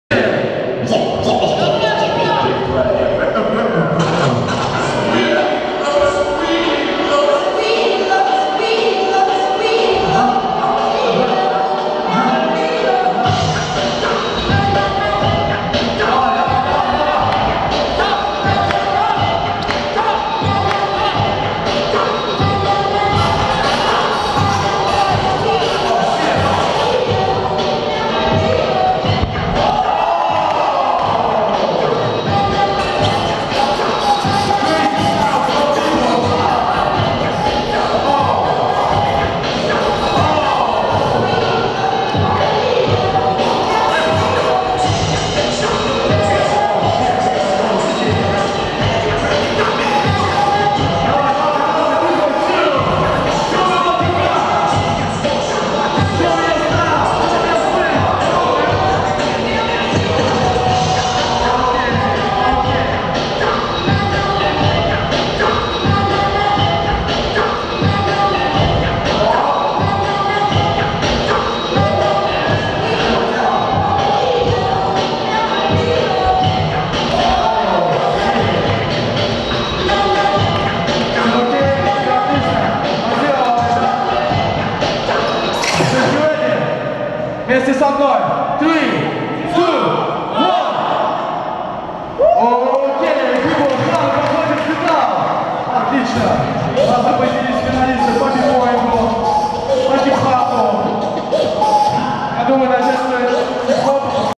Cheerus VS Khan | BBOYING | Underground Funky Base Caucases Qualifier 2014
Winner : Khan
(UFB) Underground Funky Base Caucasus Qualifier 2014(Georgia, Azerbaijan,Dagestan) Vol.8 will be held in Azerbaijan for the first time by Floor Fanatix crew & Azbboys
Official pages:
Organizator : BBoy Seed (FFC)